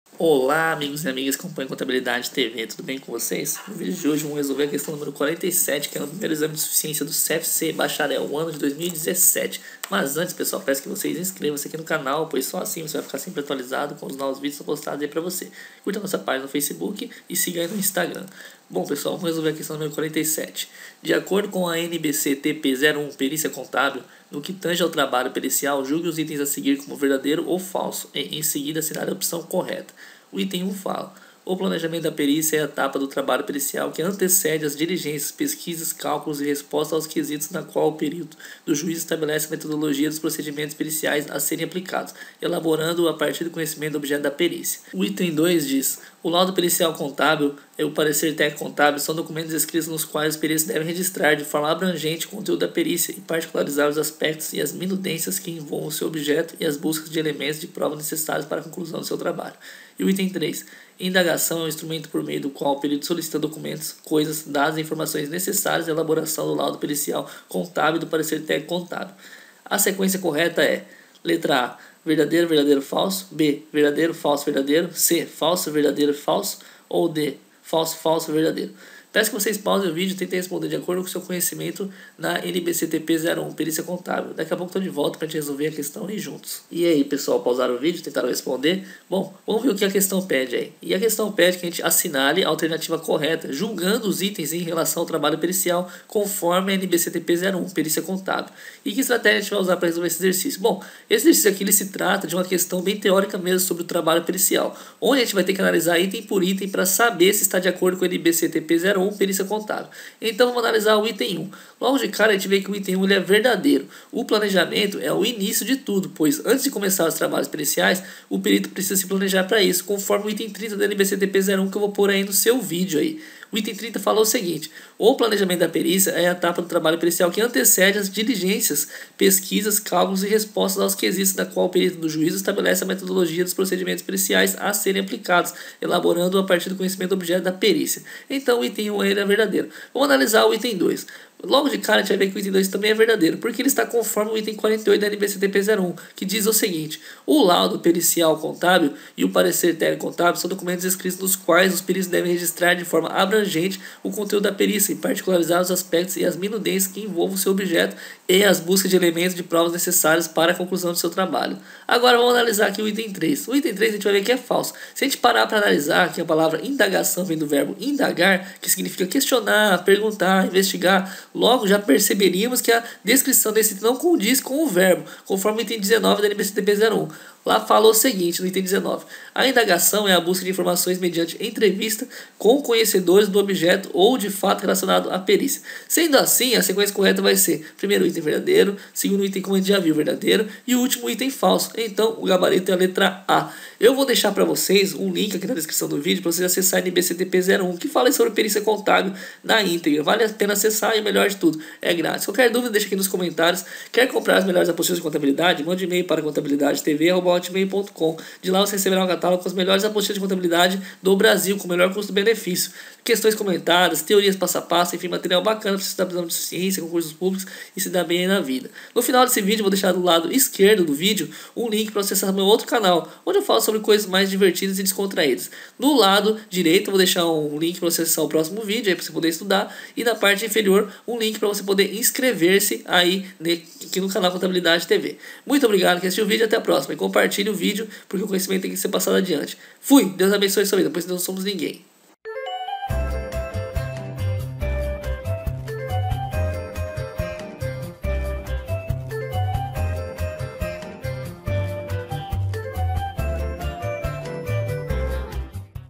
🔴 Questão 47 - Exame de Suficiência CFC CRC 2017.1 NBC TP 01 – PERÍCIA CONTÁBIL
47. De acordo com a NBC TP 01 – PERÍCIA CONTÁBIL, no que tange ao trabalho
pericial, julgue os itens a seguir como Verdadeiros (V) ou Falsos (F) e, em
seguida, assinale a opção CORRETA.
I. O planejamento da perícia é a etapa do trabalho pericial que antecede as
diligências, pesquisas, cálculos e respostas aos quesitos, na qual o perito
do juízo estabelece a metodologia dos procedimentos periciais a serem
aplicados, elaborando-o a partir do conhecimento do objeto da perícia.
II. O laudo pericial contábil e o parecer técnico-contábil são documentos
escritos, nos quais os peritos devem registrar, de forma abrangente, o
conteúdo da perícia e particularizar os aspectos e as minudências que
envolvam o seu objeto e as buscas de elementos de prova necessários
para a conclusão do seu trabalho.
III. Indagação é o instrumento por meio do qual o perito solicita
documentos, coisas, dados e informações necessárias à elaboração do
laudo pericial contábil e do parecer técnico-contábil.
A sequência CORRETA é:
a) V, V, F.
b) V, F, V.
c) F, V, F.
d) F, F, V.